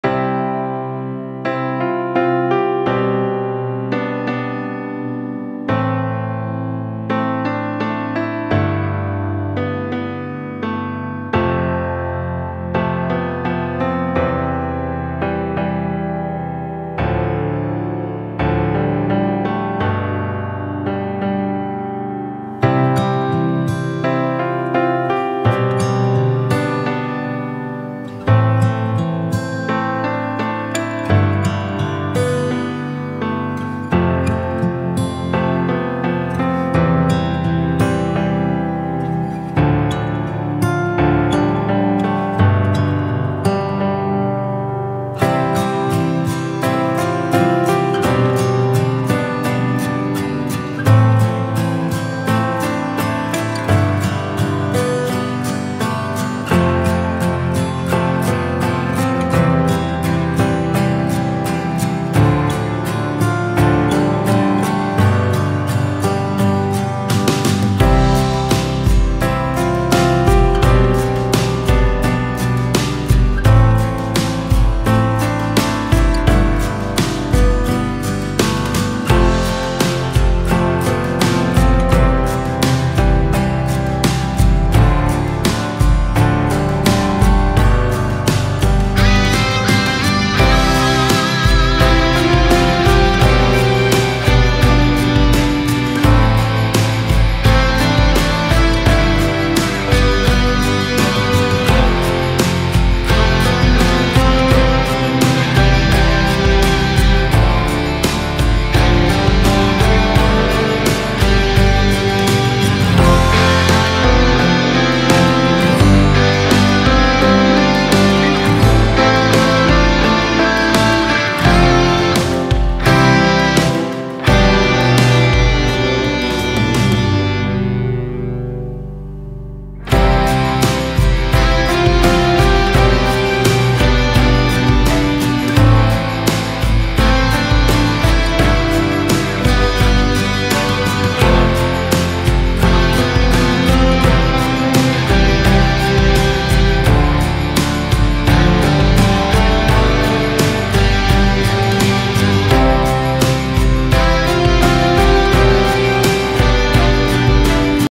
【 Speedpaint 】Frank the dragon by Jue Arts
Speedpainting Video of Frank the dragon

Don't copy, repost or trace without my written permission!

【 Tools / Facts 】
Program: Procreate, Lightroom
Tablet: iPad Air 3
Time: about 5h
Reference: Yes
Used brush for scale: From the fantastic Genevieve